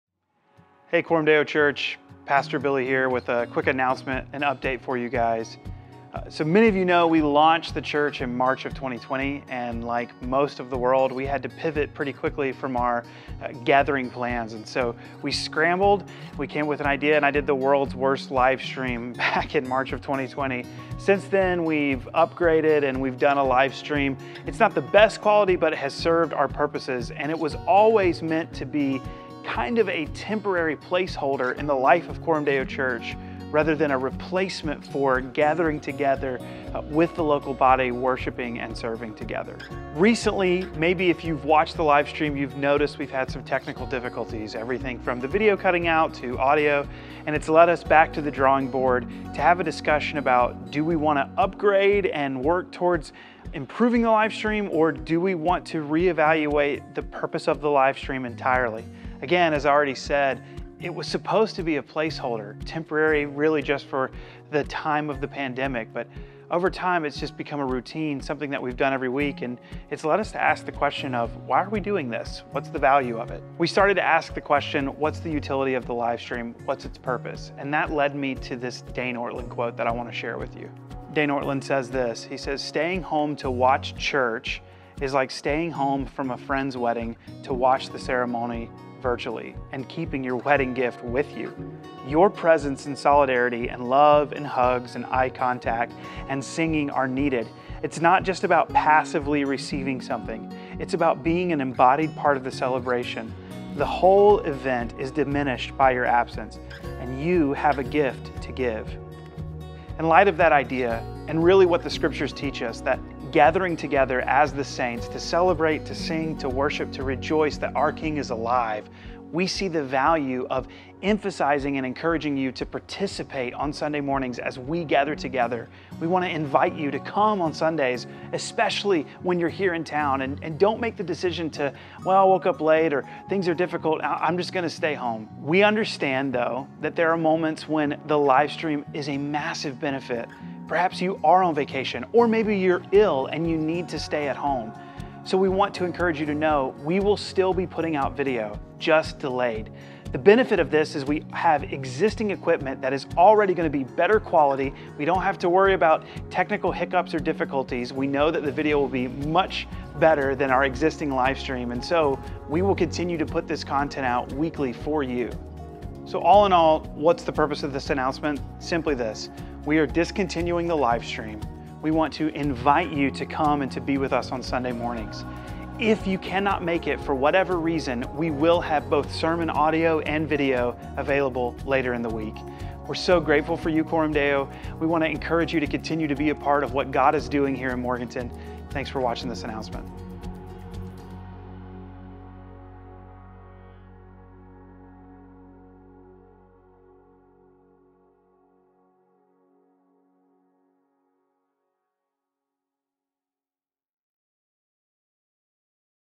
An update from Pastor Billy.
Lead Pastor, Billy Glosson shares an important update regarding the live stream.
Join us on Sundays at 9 AM. Location: 221 Herron St, Morganton, NC 28655. Visit coramdeonc.com for more information.